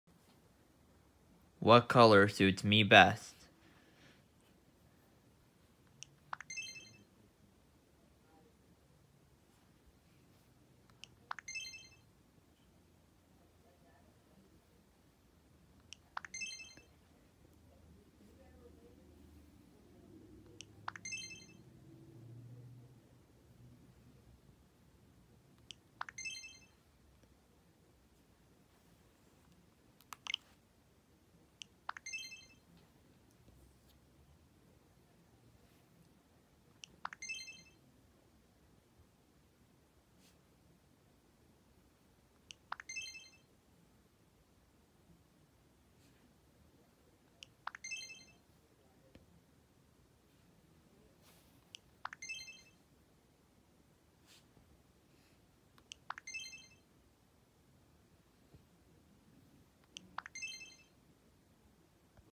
Which Color Suits Mii Best?
My Opinion:
Red: Great
Orange: Not Really
Yellow: Eh
Light Green: Kinda
Green: Good
Blue: Good
Light Blue: Fine
Pink: Eh
Purple: Cool
Brown: No thanks...
White: Good
Black: Best